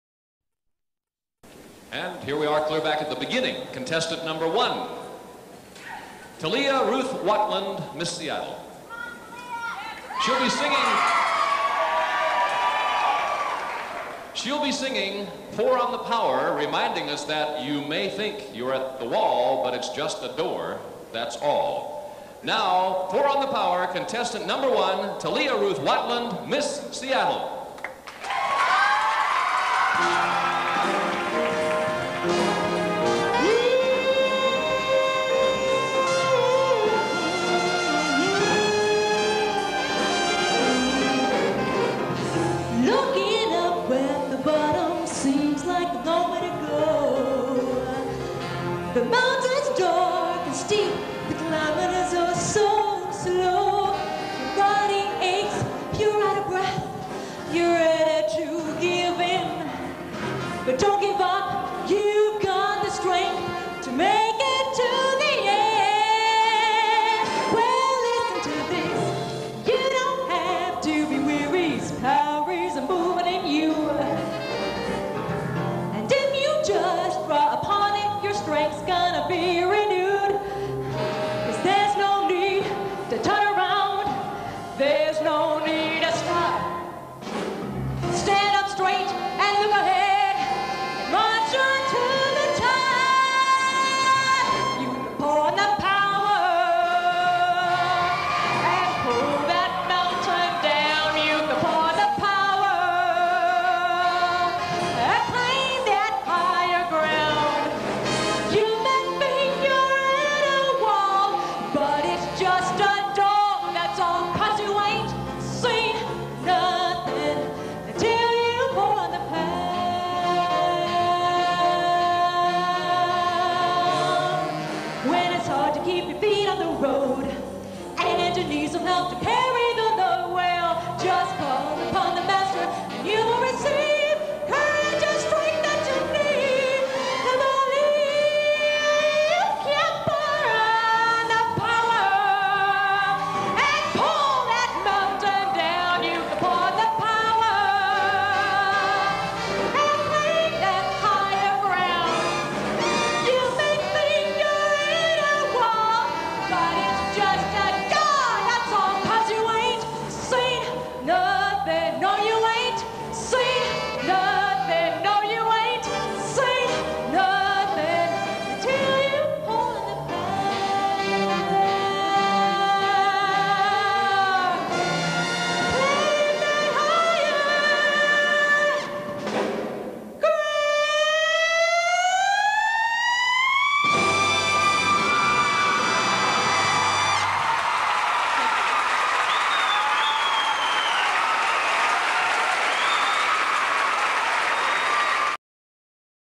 Miss Washington Pageant 1988 Talent Performance
Miss Washington Pageant 1988 Miss Seattle Talia Watland performs in talent.